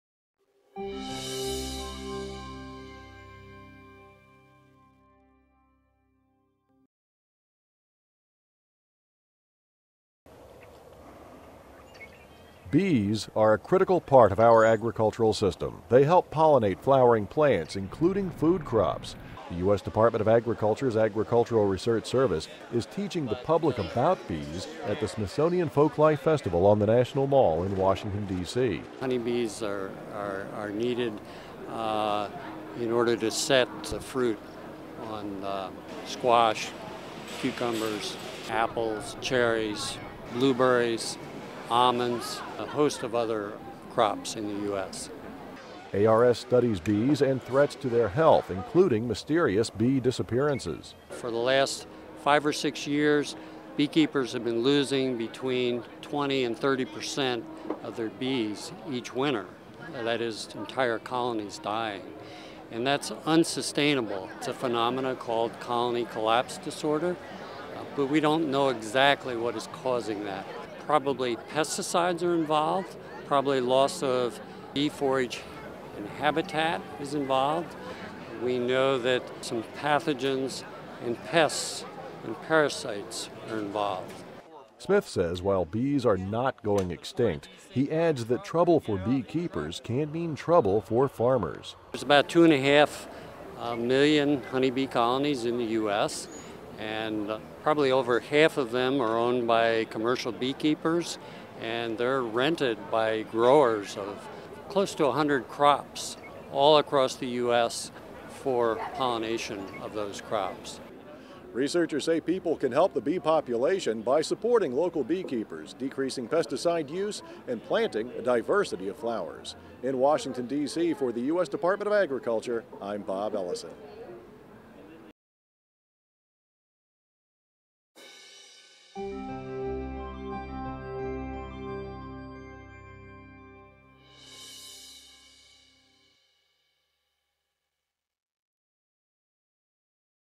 Keeping Bees Buzzing And Busy
USDA researchers are working to help people learn about bees while trying to figure out why some bee colonies are disappearing.